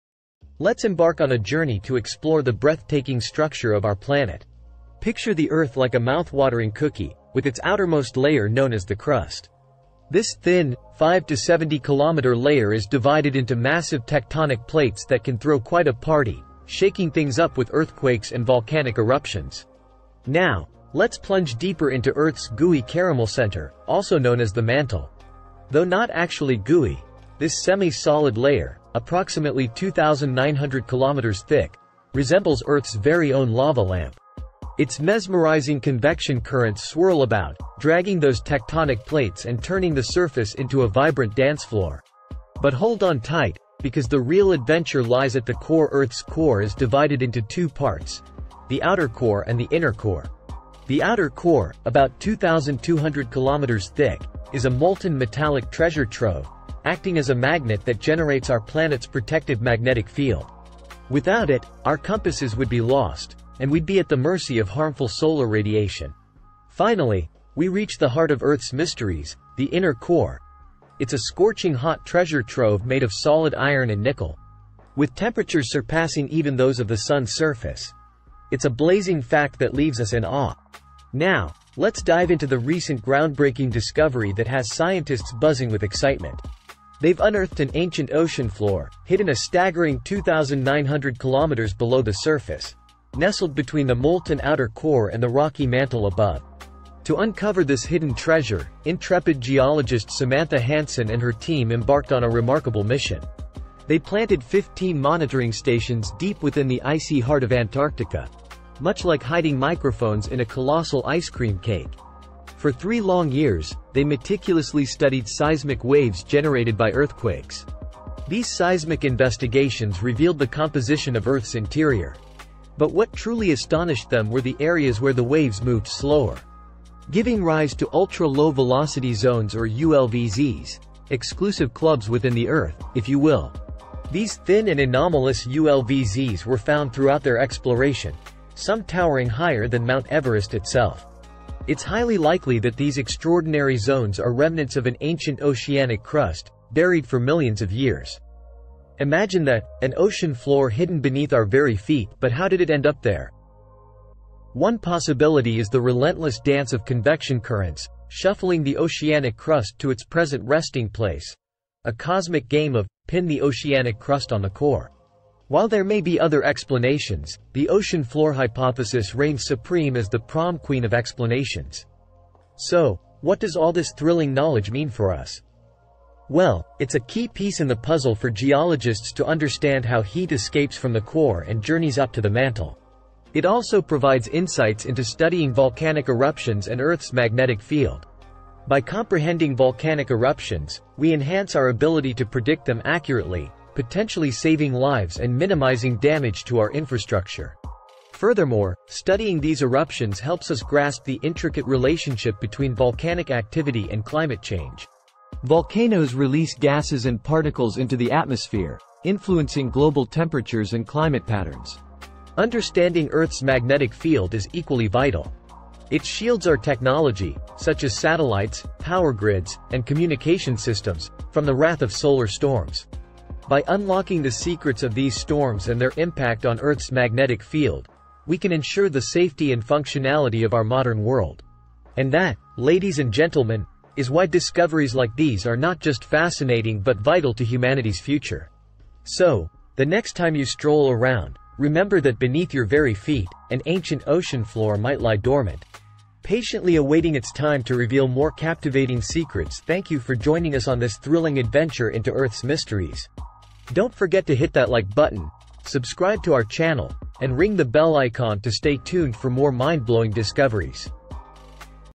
Scientists Find Ocean Floor Between Earth's Mantle and Outer Core
Join us as we dive into the mysteries of our planet and explore its fascinating structure.

In this captivating video, we'll take you on a visual expedition through Earth's layers, starting from the crust, which we liken to a delicious cookie exterior. We'll delve into the mantle, Earth's gooey caramel center, and explore the swirling currents that make our planet's surface a dynamic dance floor.

Prepare to be amazed as we venture even deeper into the core, unraveling its secrets layer by layer. We'll encounter the molten metallic outer core, Earth's secret stash of liquid metal, and learn about its crucial role in generating our planet's magnetic field.

But that's not all! Our scientific adventure doesn't end there. We'll unveil the solid iron and nickel treasure trove of Earth's inner core, with temperatures hotter than the surface of the Sun. These core-blazing facts will leave you astounded!

Now, get ready for the most exciting part. We'll reveal the groundbreaking discovery of an ancient ocean floor, hidden deep beneath the Earth's southern hemisphere. Geologists and their seismic investigations have created the most detailed map of our planet ever, uncovering a complex structure far beyond our expectations.

You'll be enthralled by the fascinating story of how 15 monitoring stations were buried in the ice of Antarctica, allowing scientists to study seismic waves from earthquakes and unlock the composition of Earth's interior. The discovery of ultra-low velocity zones (ULVZs) will leave you awe-struck, with some of them towering taller than Mount Everest!

We'll explore the captivating theory that these ULVZs are remnants of an ancient oceanic crust, buried for millions of years. We'll delve into the exciting possibilities of how the ocean floor found its current resting place and its potential impact on our understanding of Earth's geological history.

But it doesn't stop there. This discovery has profound implications for our world. We'll delve into how understanding the movement of heat from the core to the mantle can aid in predicting volcanic eruptions, saving lives, and mitigating damage to infrastructure. We'll also discuss the crucial role of Earth's magnetic field in safeguarding our technology and its potential impact on climate change.

Together, let's unlock the wonders of our planet and deepen our appreciation for the hidden treasures beneath our feet.